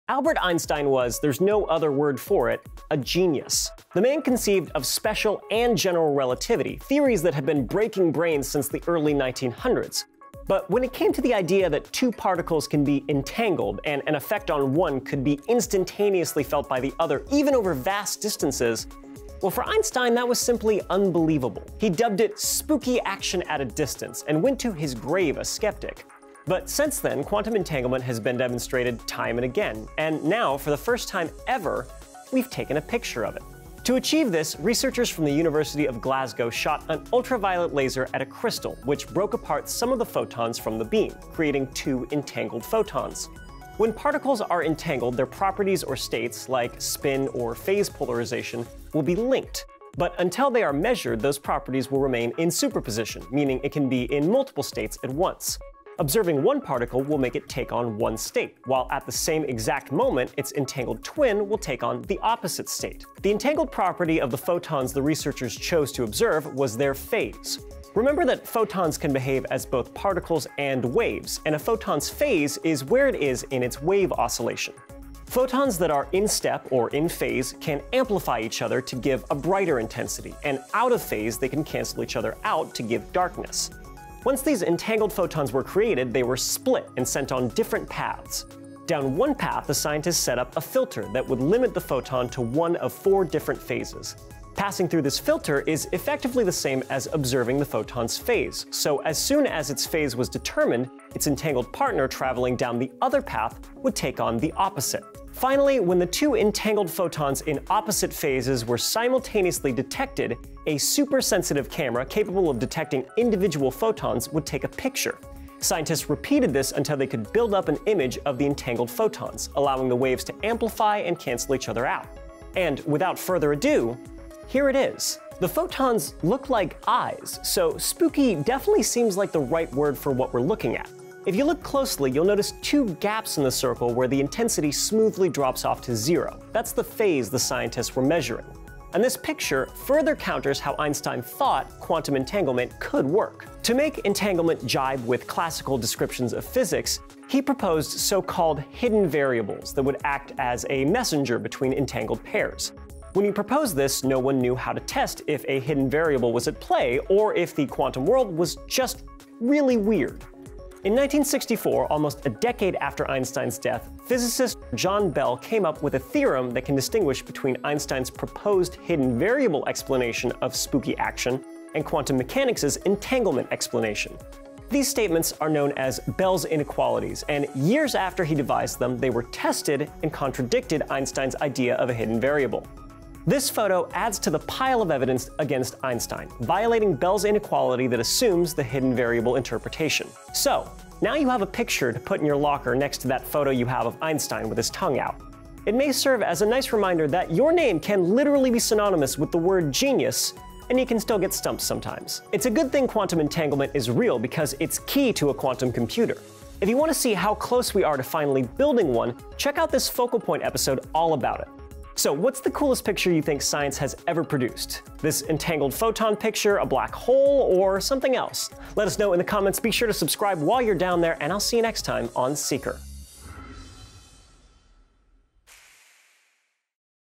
The World’s First Photo of Quantum Entanglement Could Disprove Einstein’s Theory
Einstein dubbed the idea of quantum entanglement as "spooky action at a distance." Now for the first time ever, scientists have taken a picture of it.

Today we understand quantum entanglement as when a pair of particles that cross paths and interact with each other can become connected and stay that way, even when the particles are spaced very far apart.

Once particles are intertwined in this way, changes to one particle can immediately shape the other particle, an odd scientific phenomenon that has been proven through experiments with atoms and molecules, and more recently through entangled objects of even larger scales.

Quantum entanglement is a key part of quantum mechanics, which forms the basis for fields such as quantum computing and cryptography, so there is considerable interest in advancing our understanding of it.

For scientists at the University of Glasgow, this led them to study a form of quantum entanglement known as Bell entanglement, described by late physicist John Stewart Bell.

Albert Einstein conceived of special and general relativity, but when it came to the idea that two particles can be entangled, and an impact on one particle could be instantaneously felt by the other particle, even over vast distances, for Einstein that was simply unbelievable.

Einstein even went to his grave a skeptic, but since then, quantum entanglement has been demonstrated time and time again, and now for the first time ever,  researchers from the University of Glasgow have taken a picture of it.

Learn more about these photographed entangled photons and quantum entanglement on this episode of Elements.

A 'spooky' effect of physics that Einstein couldn't believe has been photographed for the first time
"The picture represents the first photograph made by quantum entanglement, or the "spooky" pairing of particles. 'The image we've managed to capture is an elegant demonstration of a fundamental property of nature, seen for the very first time in the form of an image.'"

"A BBO crystal pumped by an ultraviolet laser is used as a source of entangled photon pairs.The two photons are separated on a beam splitter (BS). An intensified camera triggered by a SPAD is used to acquire ghost images of a phase object placed on the path of the first photon and nonlocally filtered by four different spatial filters that can be displayed on an SLM (SLM 2) placed in the other arm."

"Recently, a group at the University of Glasgow used a sophisticated system of lasers and crystals to capture the first-ever photo of quantum entanglement violating one of what's now known as 'Bell's inequalities.'"